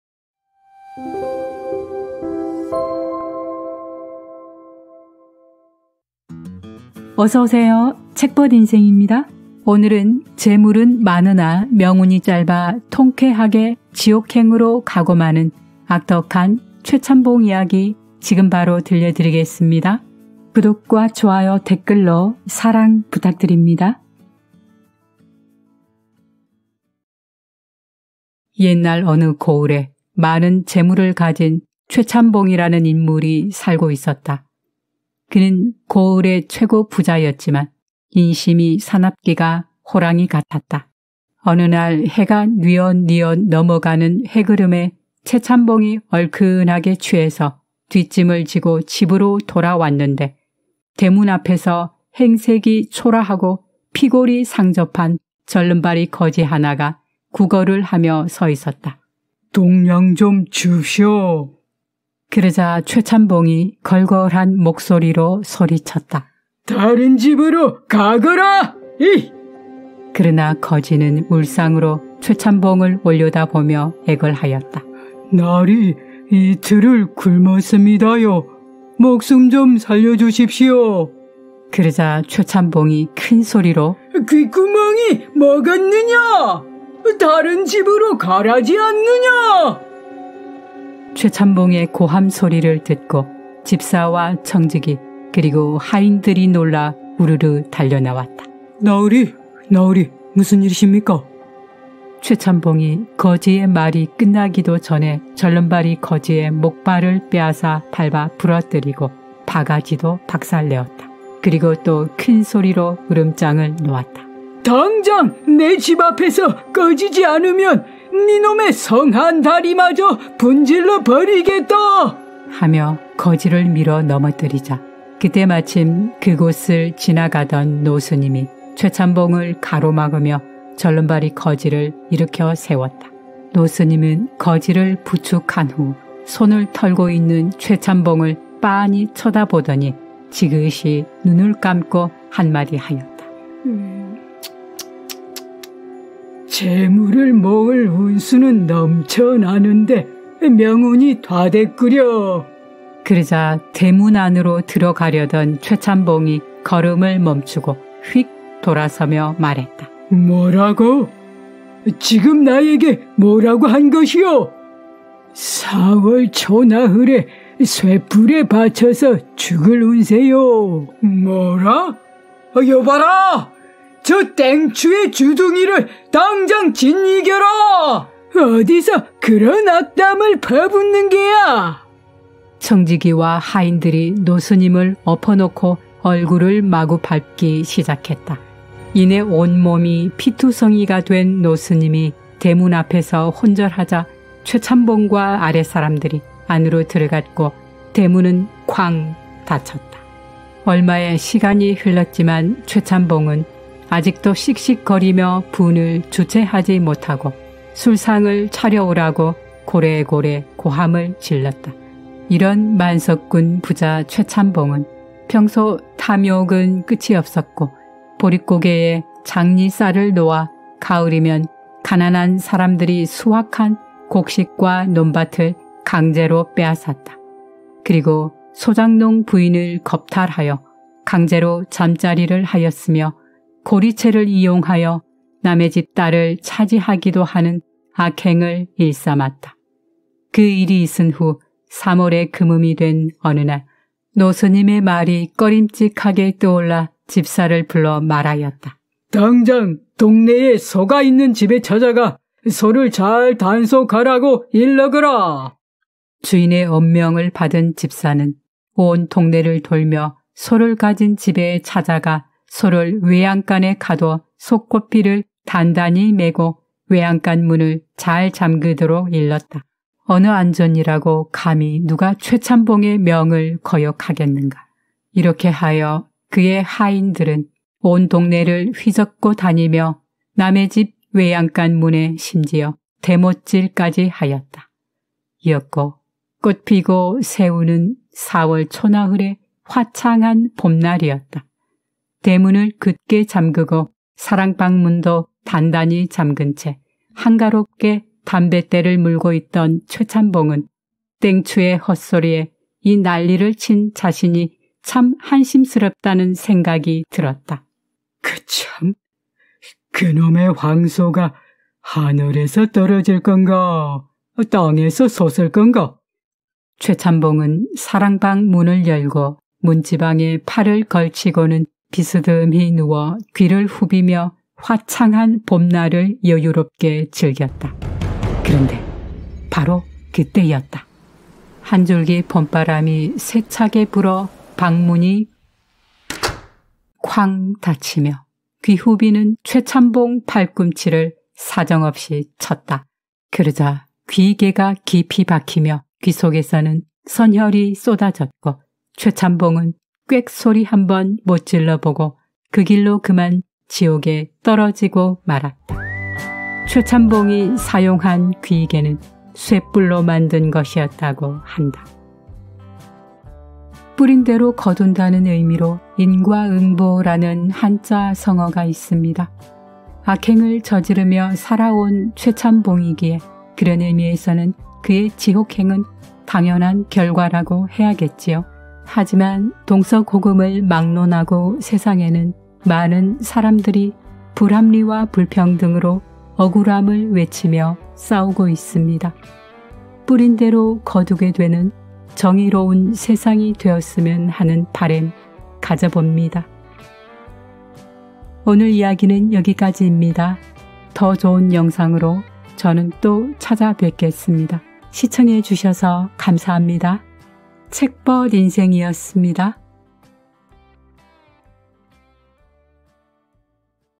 Ep. 54 설화 ⎮어떻게든 죽을 놈(만석꾼인 악덕 최참봉이 얼큰하게 취해 들어오다가 거지를 만나는데...)⎮민담, 야담, 재미있는 옛날이야기. 책벗인생
오늘  이야기는 흥미롭고 의미있는 메시지가 되어줄 고전 유머 한 편을 올려봅니다.
뿌린대로 거둔다는 의미로 '인과응보'라는 한자성어가 있습니다. 악행을 저지르며 살아온 최참봉이기에 인과응보의 의미로는 당연한 결과겠지요. 그러나 동서고금을  막론하고 세상에는 많은 사람들이 불합리와 불평등으로 억울함을 외치며 지금도 싸우고 있습니다. 뿌린대로 거두는 정의로운 세상을 바래봅니다.

본 영상의 이야기는 "고전 유머"로 전해져 오는 도서의  이야기 한 편을 참조하여 운영자인 제가 직접 새롭게 각색한 내용입니다.
*본 영상의 내용은 이야기의 본질을 훼손하지 않는 범위 내에서 제 나름의 해석과 구상으로 새롭게 다듬고 각색한 2차 저작물임을 밝혀둡니다.
*영상 사용 이미지 : 운영자인 제가 직접 촬영 또는 그림으로 묘사(창작한 이미지)하였습니다.